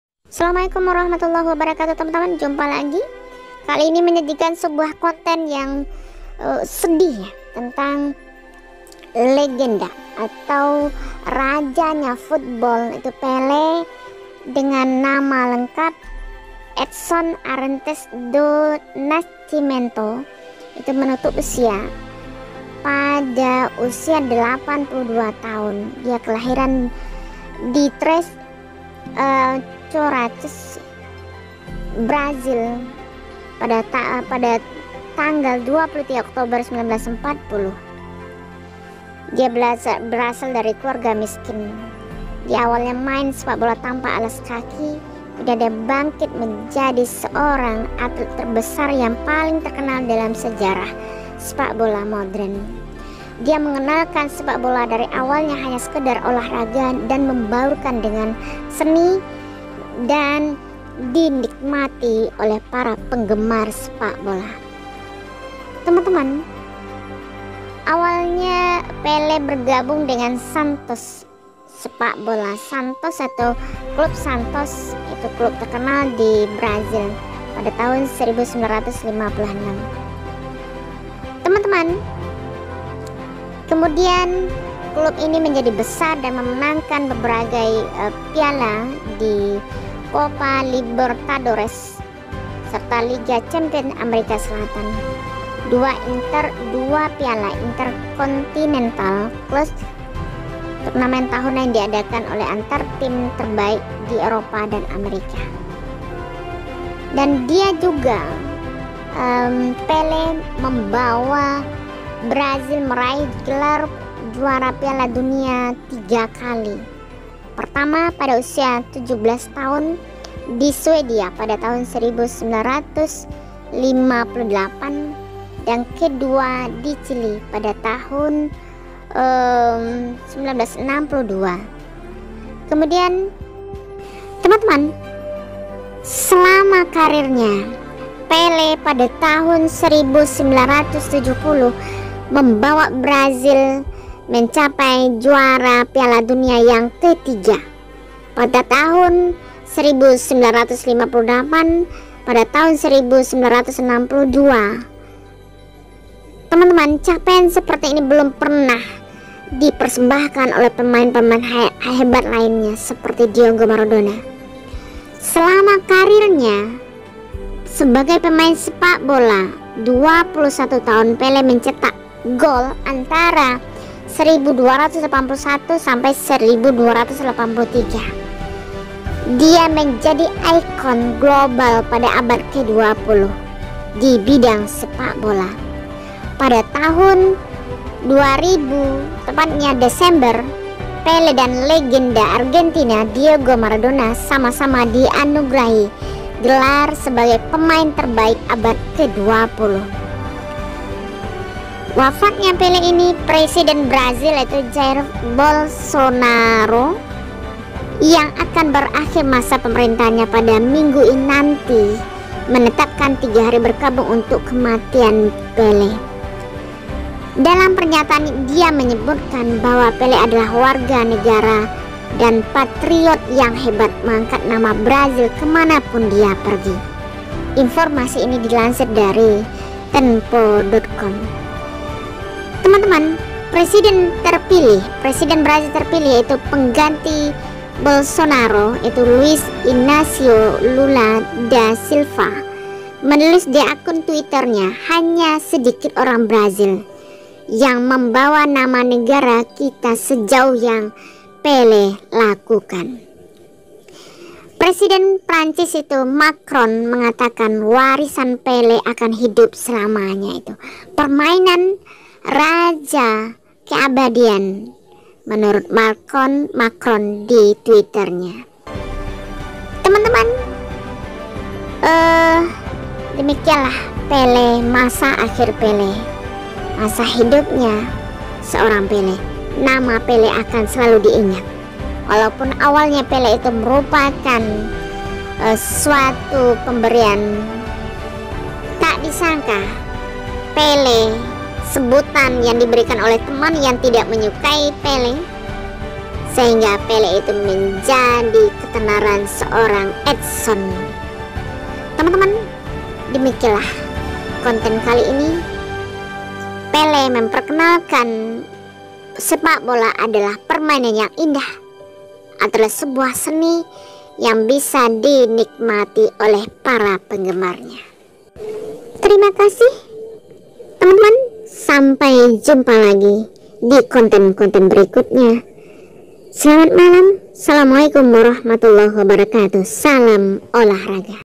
Momen Paling Sedih Dalam Sepak Bola Dunia II King Football Pele Meninggal Dunia
Momen Paling Sedih Dalam SepakBola Dunia II King Football Pele Meninggal Dunia